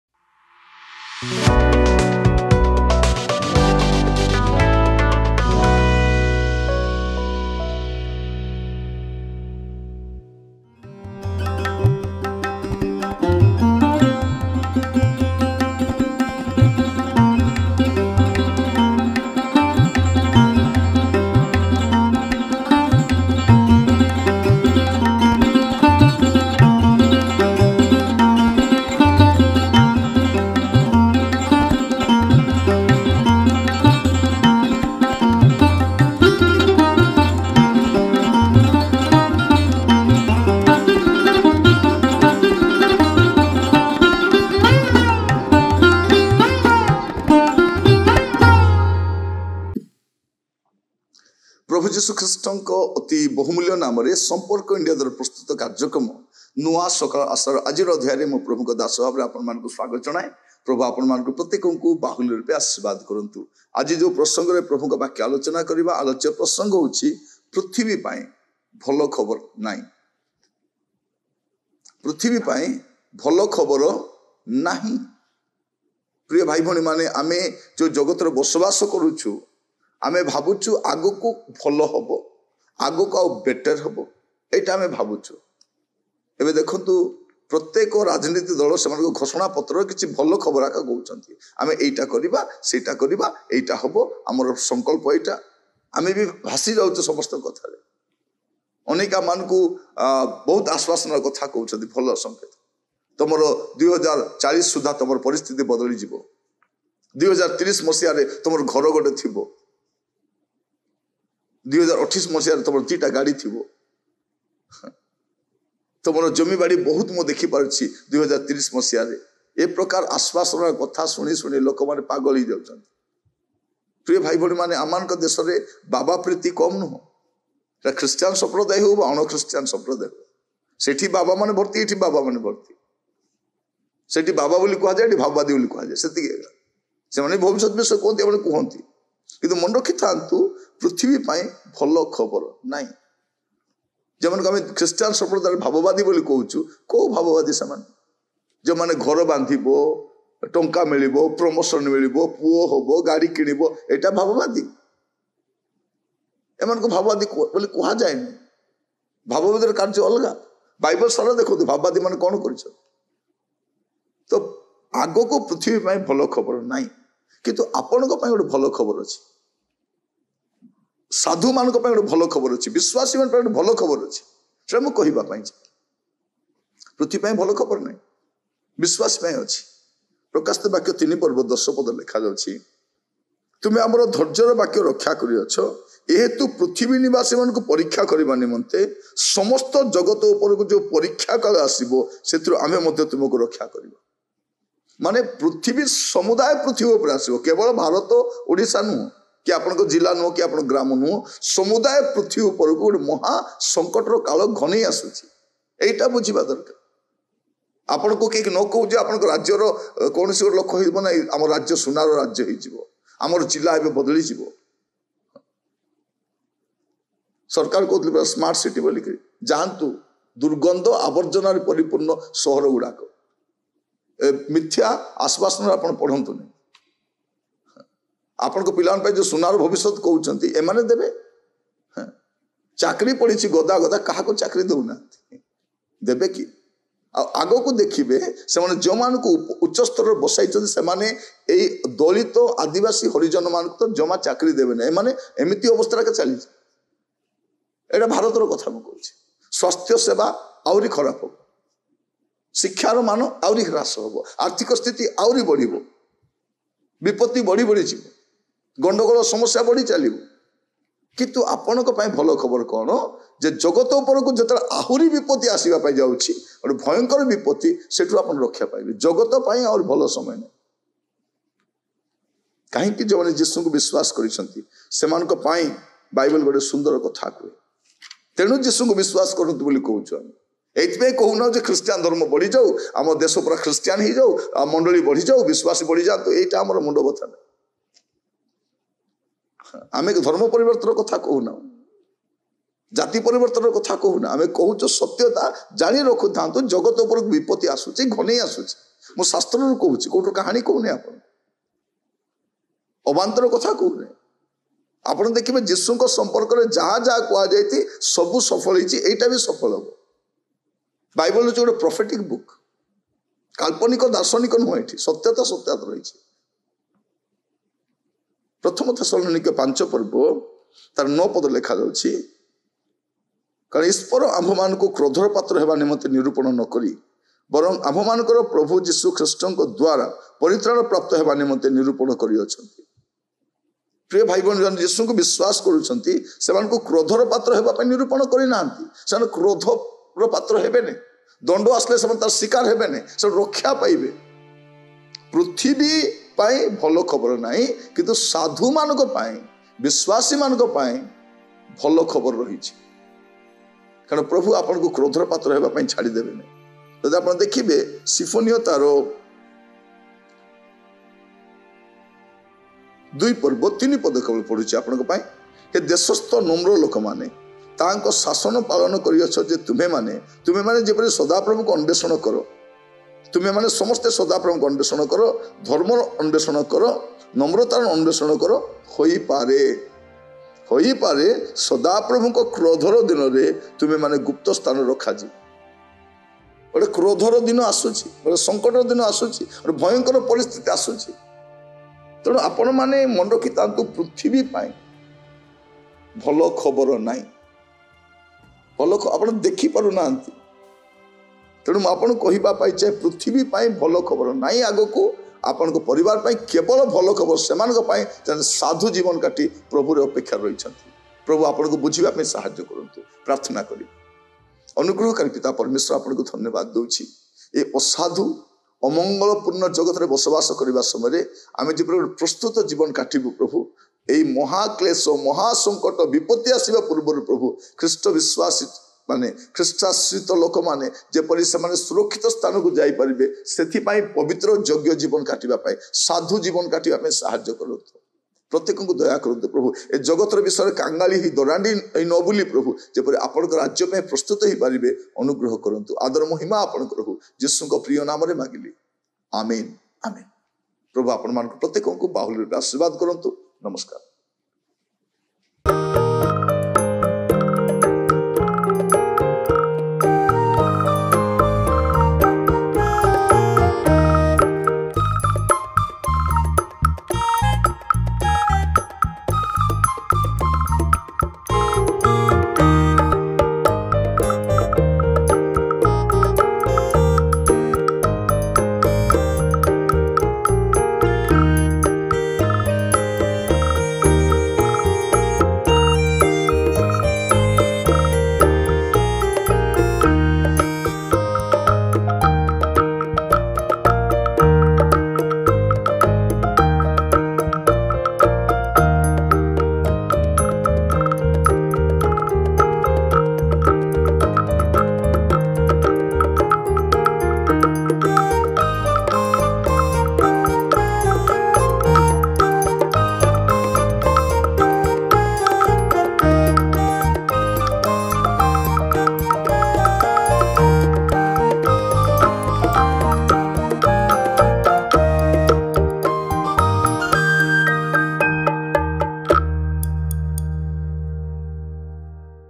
ପୃଥିବୀ ପାଇଁ ଭଲ ଖବର ନାହିଁ | ନୂଆ ସକାଳର ଆଶା | REV. NIRANJAN TURUK | SAMPARK INDIA
Thought of the day: ପୃଥିବୀ ପାଇଁ ଭଲ ଖବର ନାହିଁ
ପ୍ରକାଶିତ ବାକ୍ୟ ୩:୧୦
ତୁମ୍ଭେ ଆମ୍ଭର ଧୈର୍ଯ୍ୟର ବାକ୍ୟ ରକ୍ଷା କରିଅଛ, ଏହେତୁ ପୃଥିବୀ ନିବାସୀମାନଙ୍କୁ ପରୀକ୍ଷା କରିବା ନିମନ୍ତେ ସମସ୍ତ ଜଗତ ଉପରକୁ ଯେଉଁ ପରୀକ୍ଷାକାଳ ଆସିବ, ସେଥିରୁ ଆମ୍ଭେ ମଧ୍ୟ ତୁମ୍ଭକୁ ରକ୍ଷା କରିବୁ।